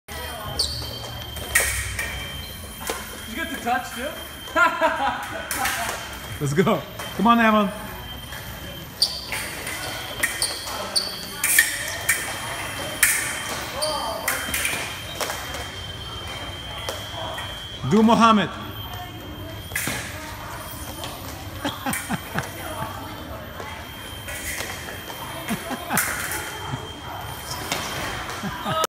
July 15, 2016
Race Imboden doing his "Muhammad Ali" thing